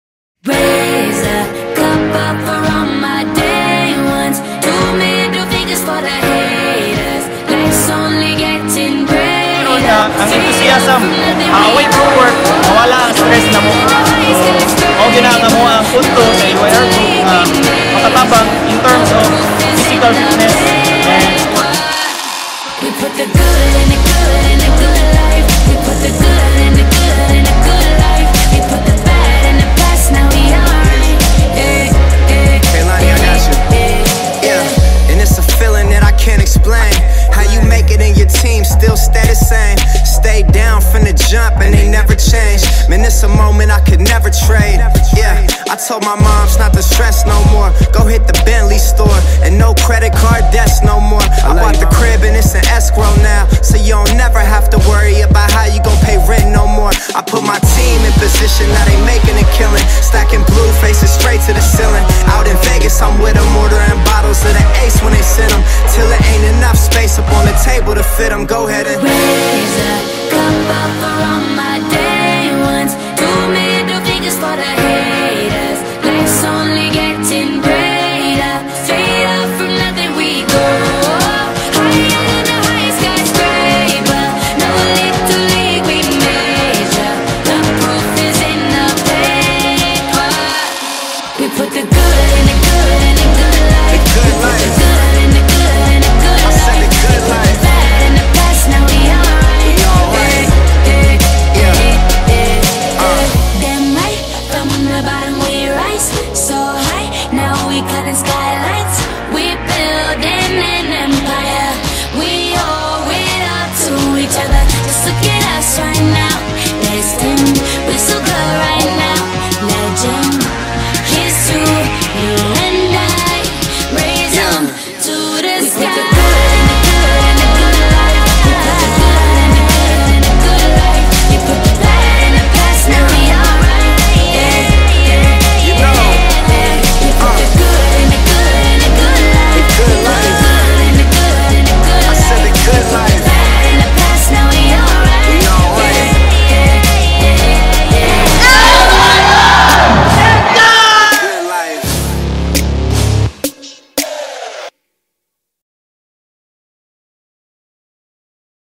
LYRLYMPICS 2017
LYR Group of Companies

Presents...

This is the LYR Family celebrating its passion for sports, fitness, and fun!

Witness LYR employees from all over Mindanao with their families compete with one another together with Suppliers and Business Partners as guest participants showcase their competitiveness and camaraderie in different sports and games.

A big thanks to everyone who participated and made this possible!
See you again next year!